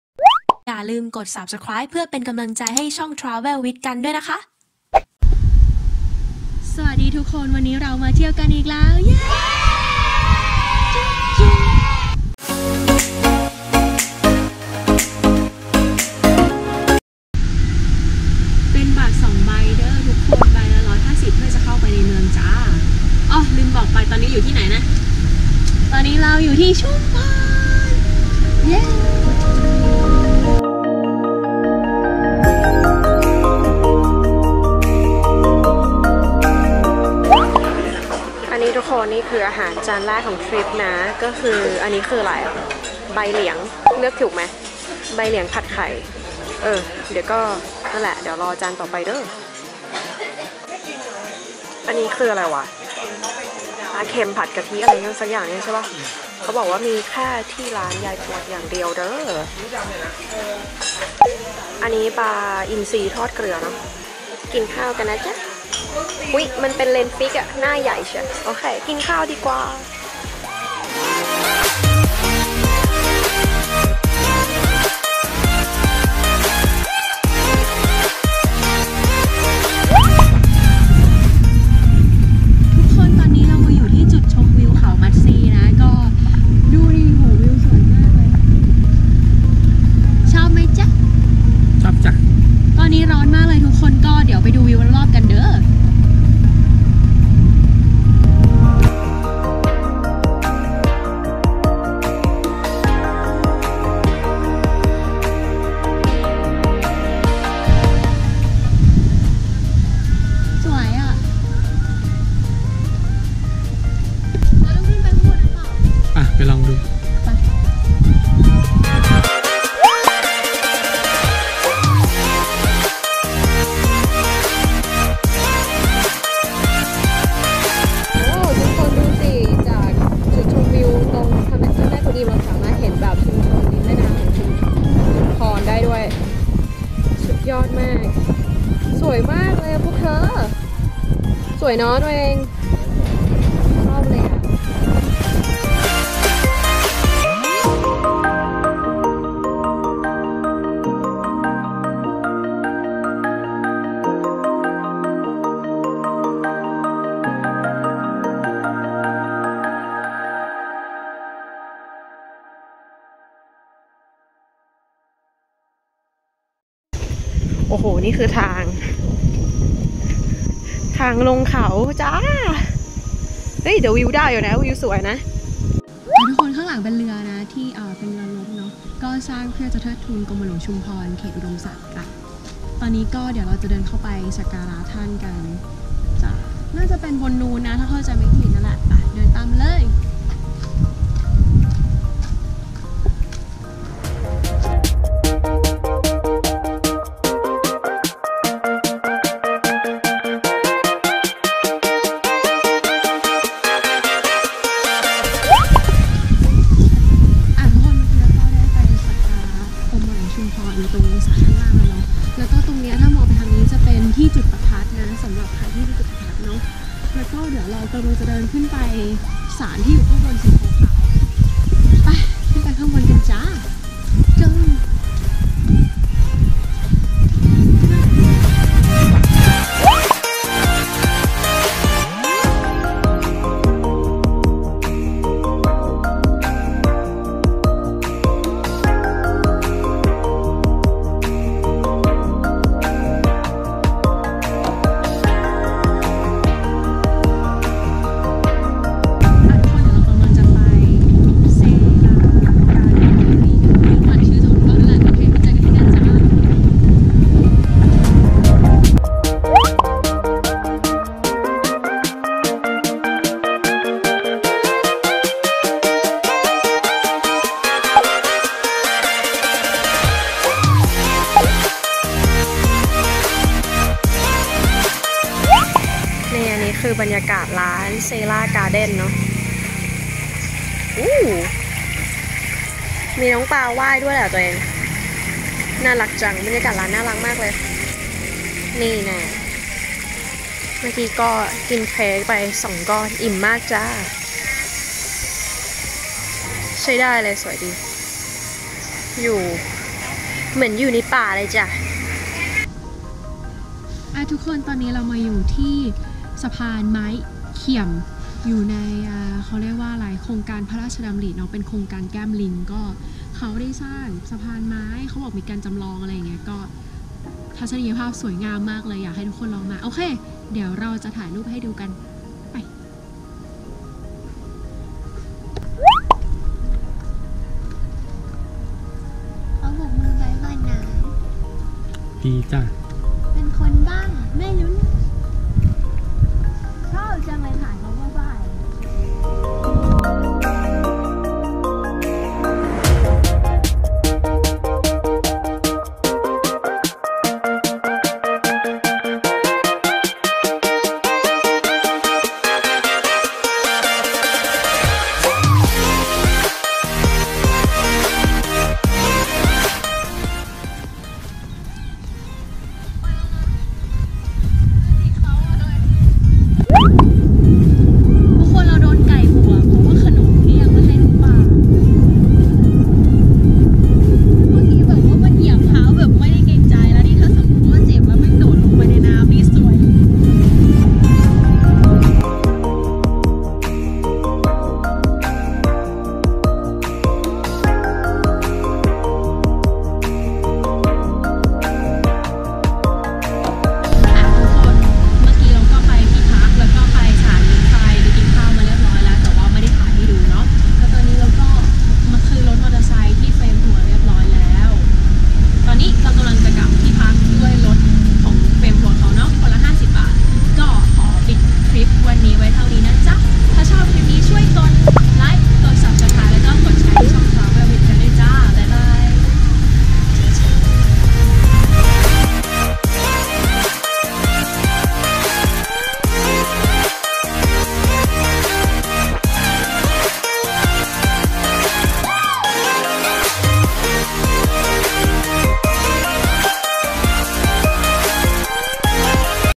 เที่ยวพร้อมกัน(ต์) ทั่วไทย EP#29 ชุมพร - เที่ยวชุมพรประตูสู่ภาคใต้ 1/
ชุมพร :) เมืองเล็กๆ ที่เป็นประตูสู่ภาคใต้ รอบนี้เดินทางด้วยนกแอร์ อื้อหืออออ รีไทม์ รัวมาก นึกว่าจะไม่ได้ไป!! แต่ในที่สุดก็ได้ไปจ้าาา เราจะอยู่ที่นี่ 3 วัน 2 คืน ดังนั้นคลิปเรามี 3 ตอนฝากด้วยน้าาา

วันแรก
- ร้านยายปวด
- เขามัทรี
- กรมหลวงชุมพร
- Cera Garden
- สะพานไม้เคี่ยม